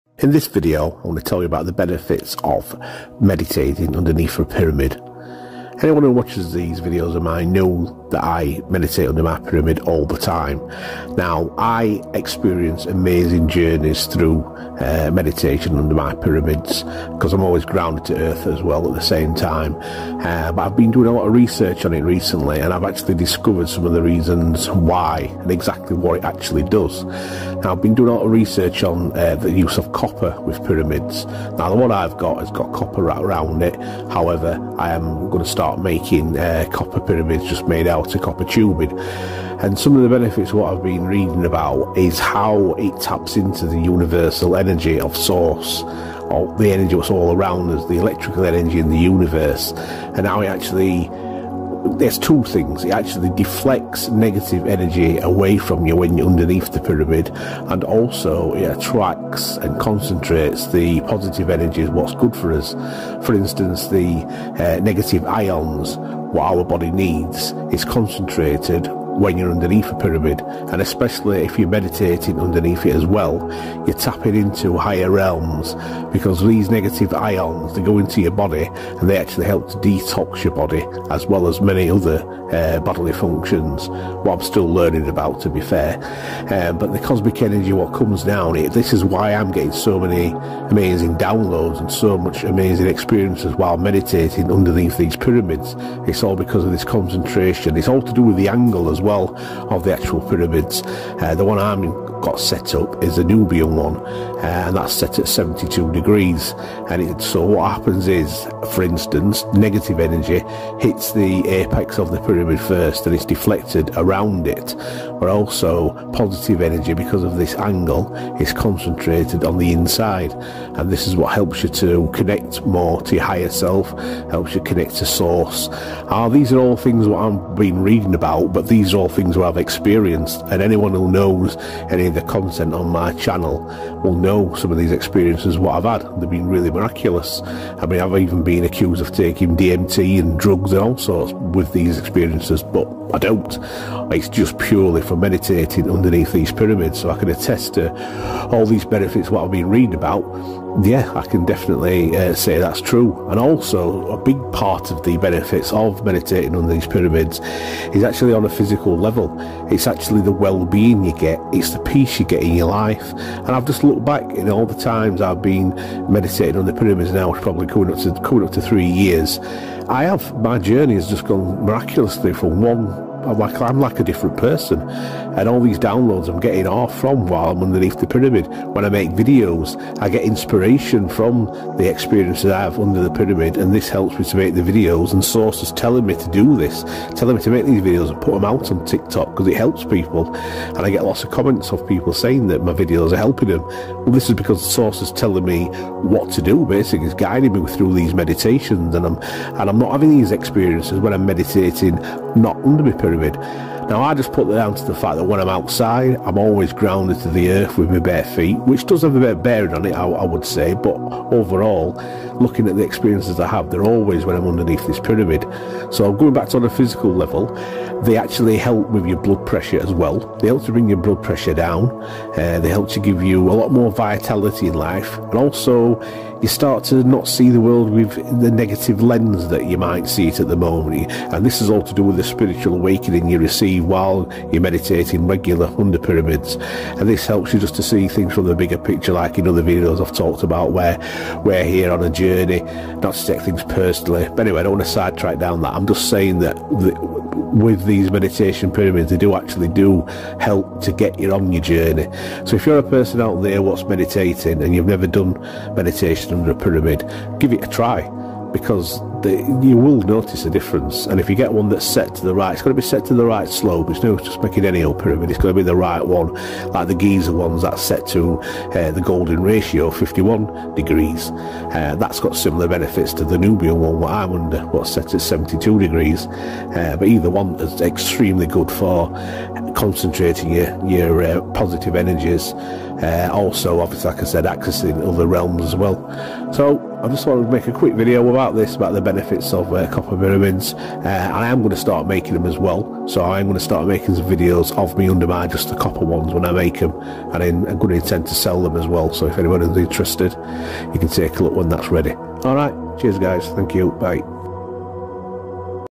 Benefits of Meditating Under a Pyramid
The benefits from meditating under a pyramid are numerous.

These include deeper, more revealing meditation journeys.

Also, the health benefits are amazing, too. In this video, I go into more details about these.